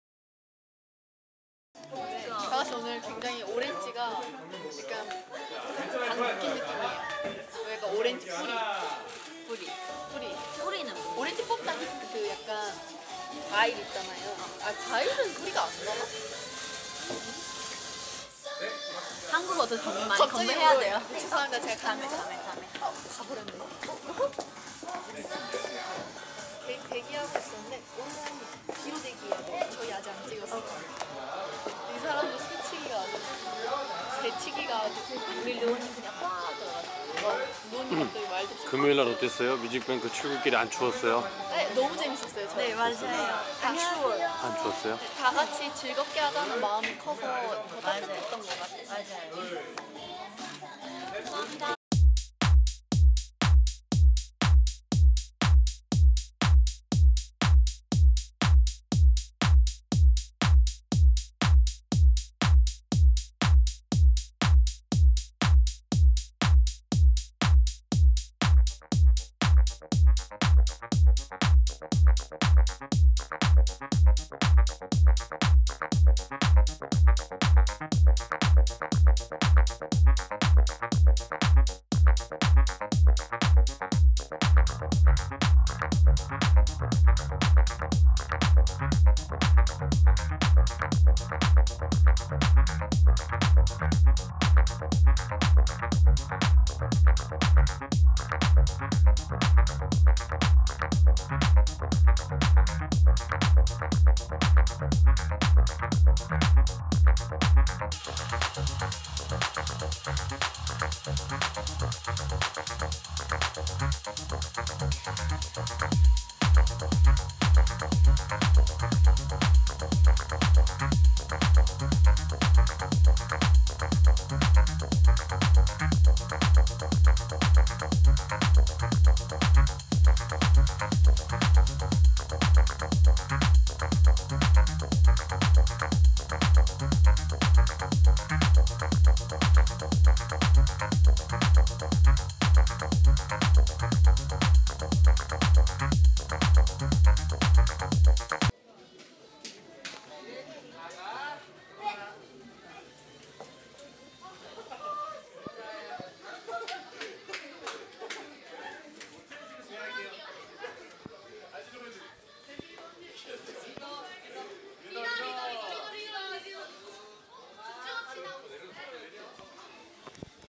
181218 4K 네이처 뮤뱅 퍼포먼스 인터뷰 tbs '팩트인스타' 녹화 전 포토타임 영상
18일 오후 서울시 마포구 상암동 tbs 오픈스튜디오에서 '팩트인스타 스타인터뷰' 네이처(Nature)편' 녹화 현장을 공개했다. 이날 뉴스인스타가 저번주 14일 금요일 뮤직뱅크 출근길에 강추위에도 불구하고 여름 패션으로 펼친 퍼포먼스에 대해 소감을 질문 4K 영상 직캠